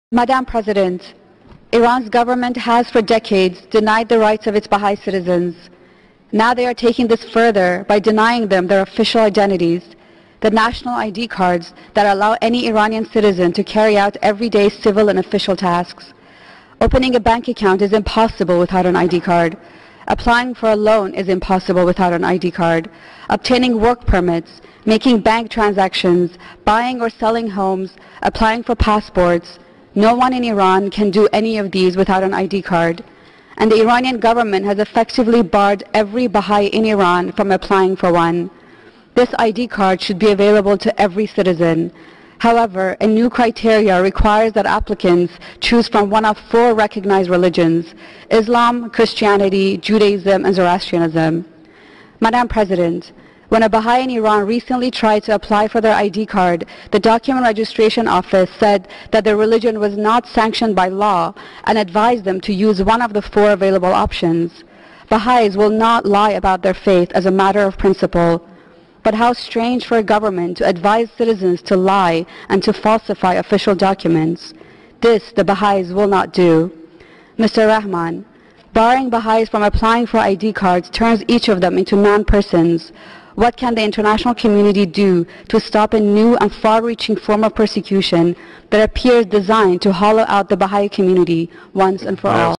بیانیه جامعه جهانی بهائی در سازمان ملل در مورد بهائیان ایران، جلسه بررسی دوره‌ای جهانی در مورد ایران
SR on Human Rights in Iran - 25th Meeting, 43rd Regular Session Human Rights Council

BIC UN Statement